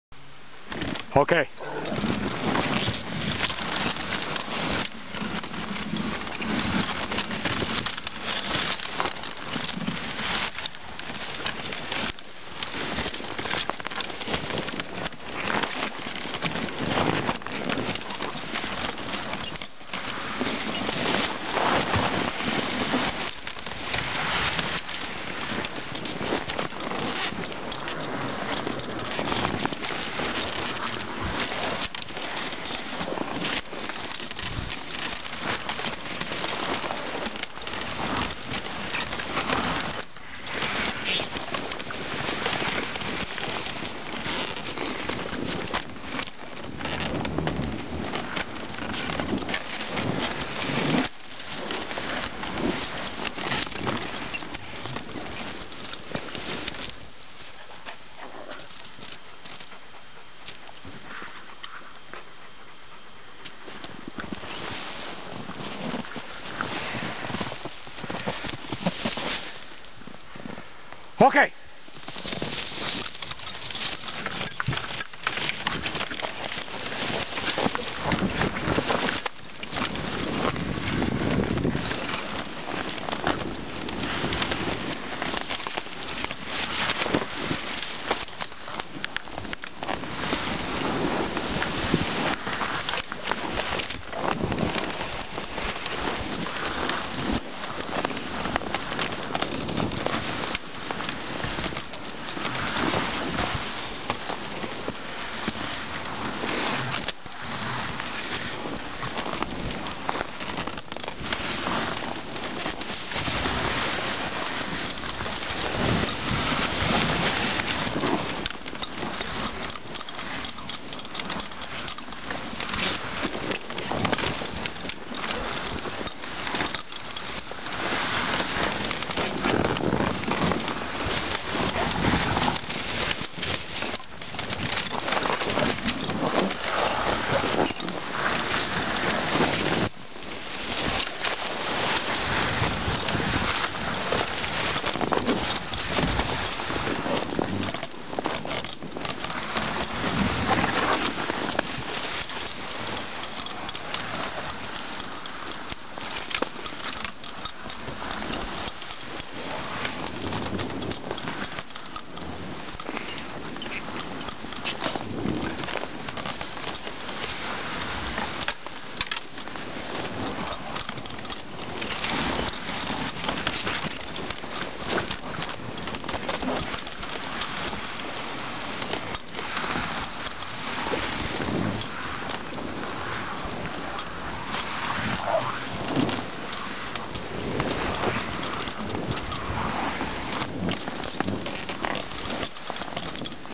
Dog Sledding With Alaskan Malalmutes III
A training run for pups running through the forest in Alaska